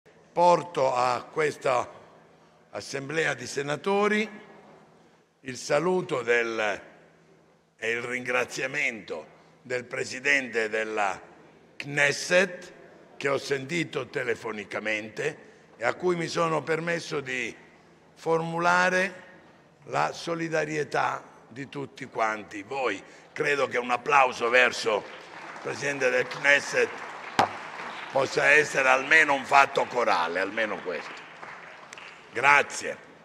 Israele, applauso del Senato al presidente della Knesset
La Russa: "L'ho sentito al telefono e ho formulato la nostra solidarietà"

POLITICA (Roma). Un applauso al presidente della Knesset da parte dell'Aula del Senato. Lo chiede il presidente Ignazio La Russa, comunicando all'assemblea di averlo appena sentito, alla presenza del vicepremier e ministro degli Esteri, Antonio Tajani, appena arrivato in Aula. "Porto all'assemblea dei senatori - sono le parole di La Russa - il saluto e il ringraziamento del presidente della Knesset che ho sentito telefonicamente e al quale mi sono permesso di formulare la solidarietà di tutti quanti voi. Credo che un applauso possa essere un fatto corale". (NPK) (Luca Piludu/alanews)

porto a questa assemblea di Senatori  il saluto del è il ringraziamento del presidente della che ho sentito telefonicamente e a cui mi sono permesso di formulare la solidarietà di tutti quanti voi credo che un applauso verso presente Del Mes possa essere meno un fatto ancora le Almeno questo grazie